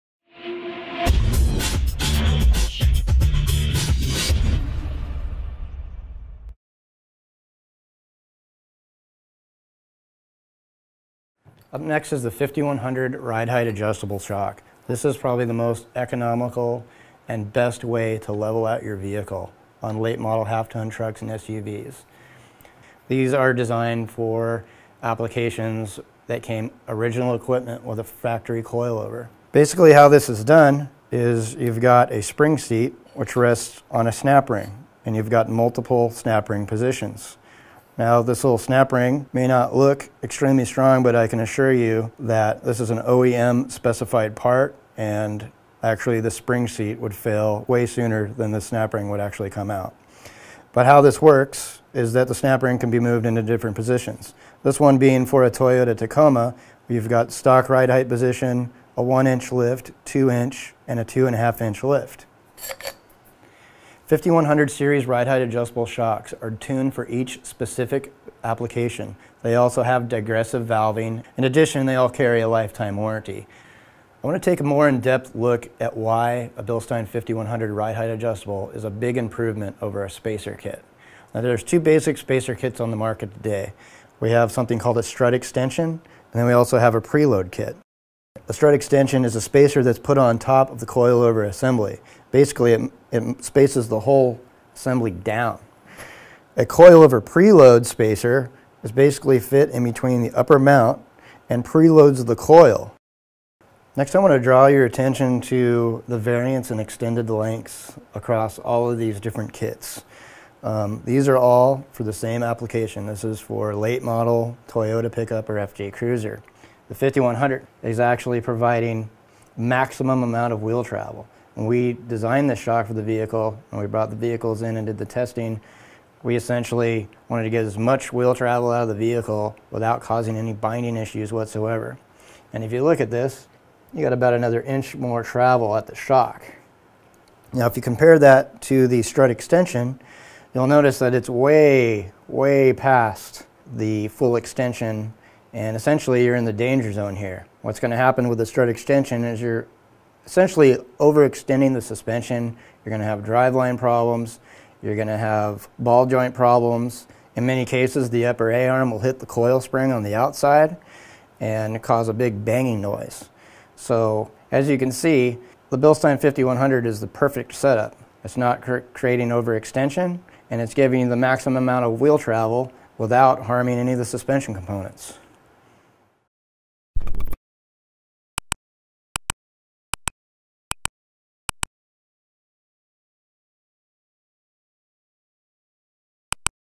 Bilstein 5100 Ride Height Adjustable Shocks
Shane from Bilstein goes over Bilstein 5100 series ride height adjustable shocks and how they are an extremely affordable and practical alternative to coil spring spacers.